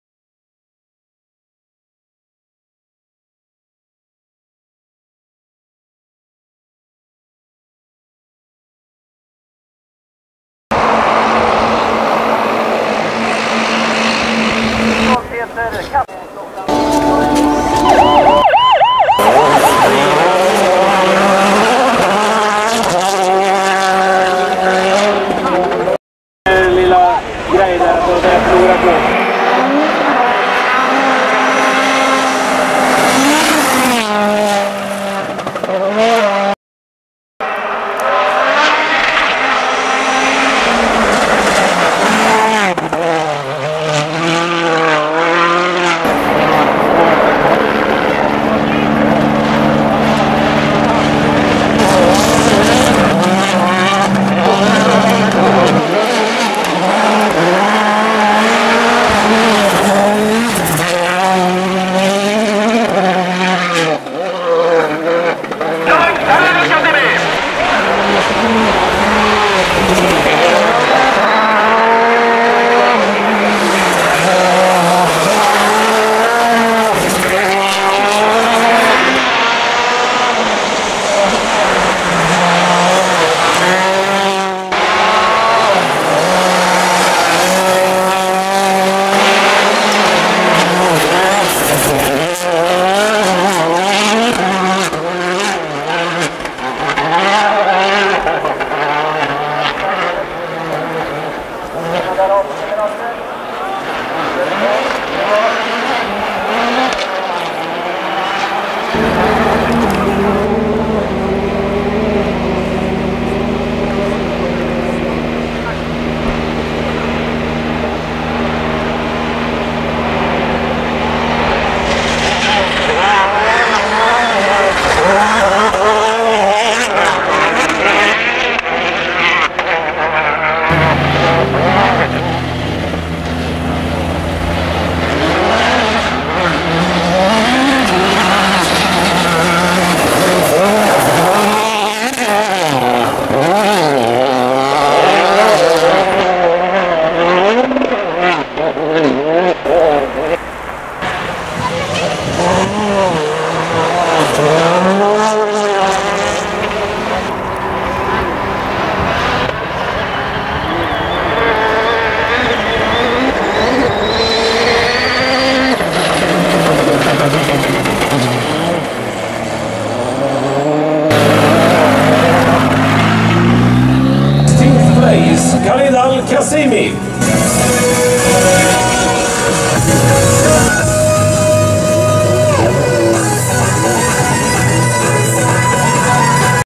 Rally Sweden 2014_Sheikh Khalid Al Qassimi
Khalid Al Qassimi: "I tried to be more aggressive in Day 3. Things went well from the second stage onwards. We had a few hairy moments but we made good progress. On the Power Stage, I preferred to ease off a little and not make a mistake, so I could be sure of reaching the end without any problems."
Video by: WRCCarlos

شارك الشيخ خالد القاسمي برالي السويد، الجولة الثانية من بطولة العالم للراليات 2014. وتمكن من تحقيق تقدم جيد. والجدير بالذكر أن استراتيجية الضغط الحذر أدت إلى عدم وقوع السائق الإماراتي في أخطاء خلال مراحل الرالي التي وصفت بالزلقة جداً. وتمكن سائق فريق سيتروين توتال أبوظبي العالمي للراليات مادس أوستبيرغ من انهاء الرالي في المركز الثالث ليتقدم فريق أبوظبي العالمي إلى المركز الثاني في ترتيب الصانعين بعد رالي السويد.
 تصوير: دبليوآرسي كارلوس